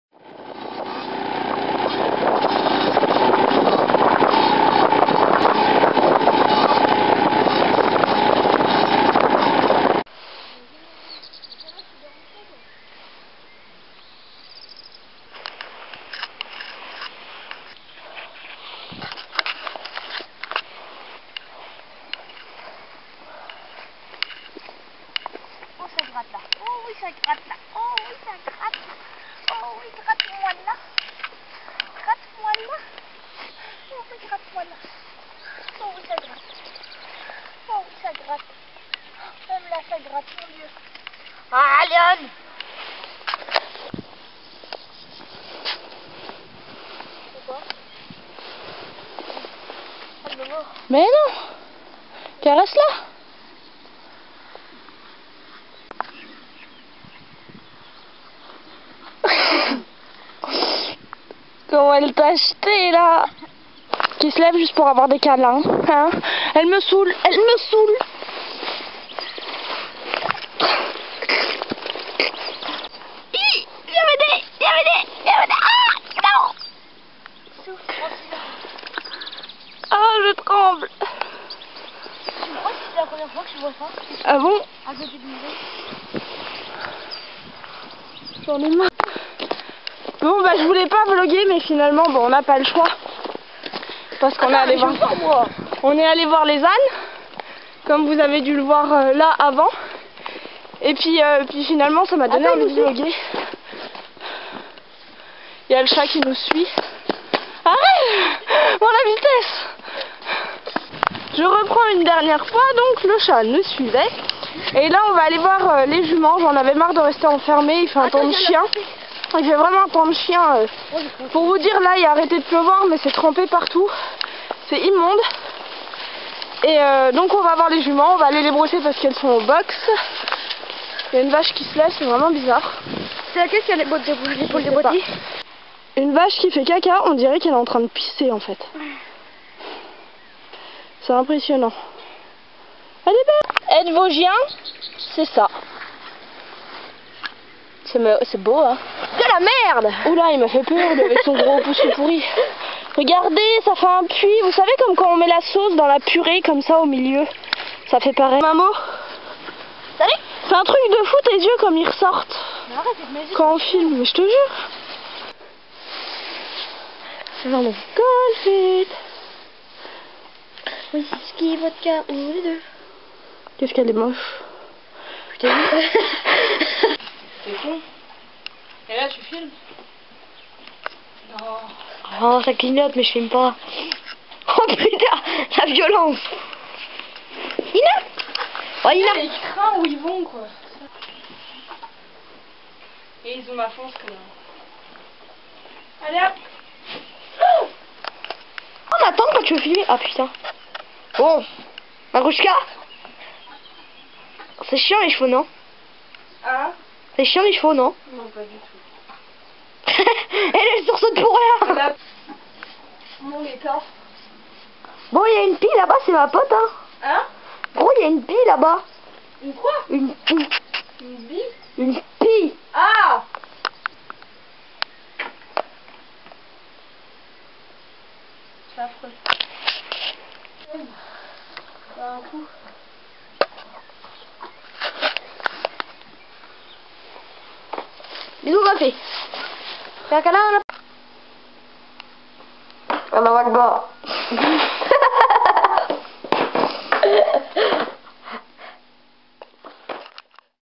[DOMAINE D'AZIA] Un moment avec les ânes !
DEROULE MOI !

Aujourd'hui était une journée simple, banale, mais pleine de tendresse tout de même ! Un nouveau vlog pour le plaisir et rien que pour cela !